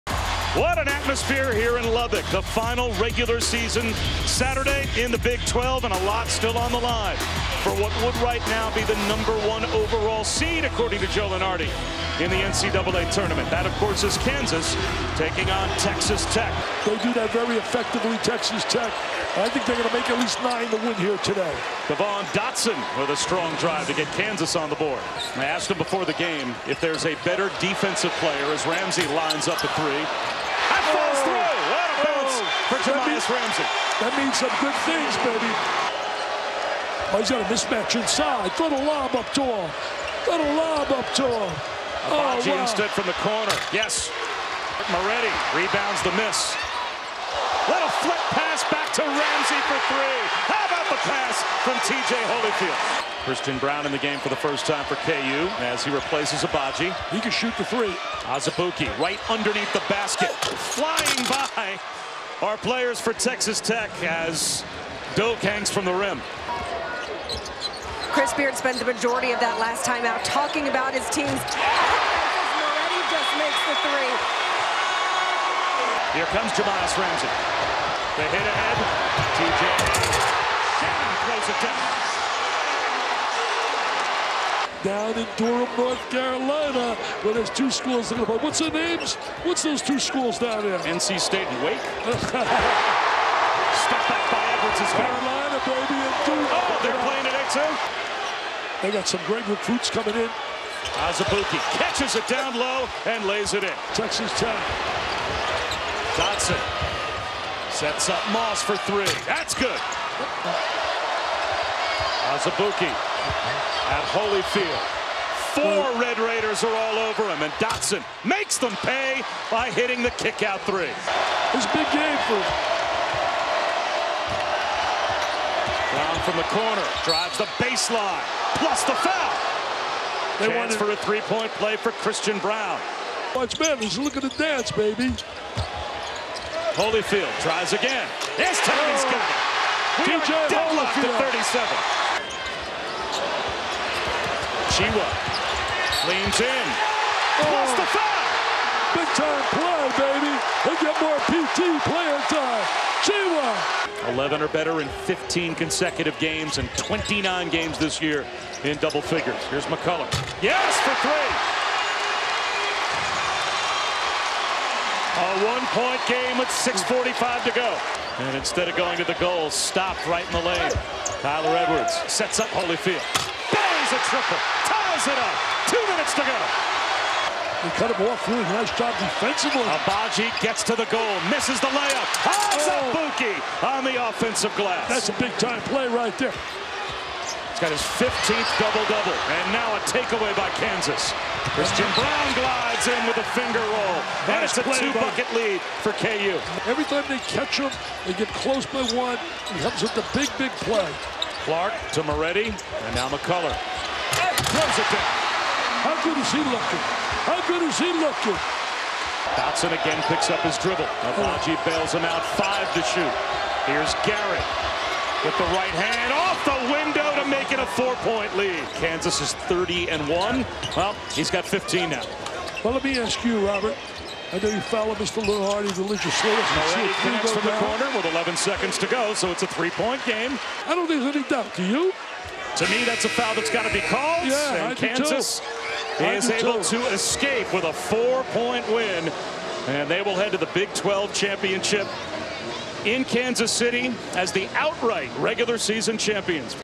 Kansas at Texas Tech Men’s Basketball Highlights
A 17-point outing from sophomore guard Devon Dotson paired with senior Udoka Azubuike’s 15th double-double of the season was just enough to push No. 1 Kansas past Texas Tech, 66-62, in the regular-season finale Saturday afternoon inside United Supermarkets Arena.